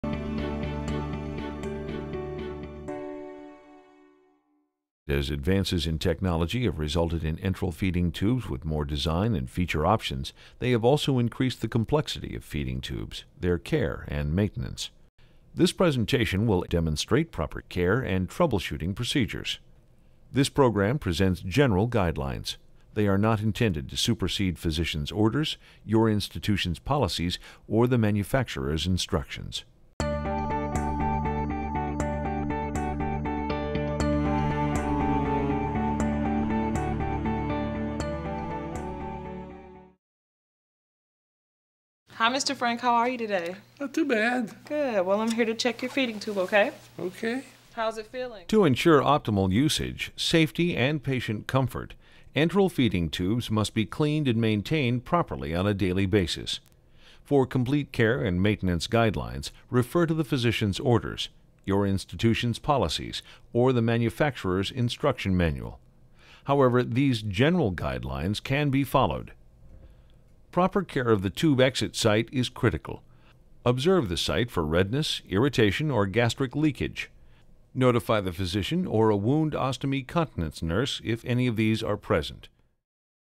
M245CS - Enteral Feeding Tubes: A Guide for Nurses Part 3: Daily Care and Troubleshooting
Enteral feeding tubes technology has also brought more design and feature options, increasing the complexity of feeding tubes. As a result, familiarity with enteral feeding tubes, and their use and care, is a useful skill for all healthcare providers. This program presents general guidelines and techniques for care and troubleshooting procedures for enteral feeding tubes.

After completing this course, the learner should be able to:
• List the steps required to properly clean and maintain various types of enteral feeding tubes.
• Identify and address signs of irritation or infection at the tube entry/exit site.
• List steps required to flush feeding tubes.
• Describe how to perform weekly maintenance on the balloon of a balloon gastrostomy tube.
• List the steps in the Daily Maintenance Checklist
• Describe troubleshooting various problems that can occur with enteral feeding tubes, including patient distress, pain, tube leakage, dislodgment and clogging of tubes.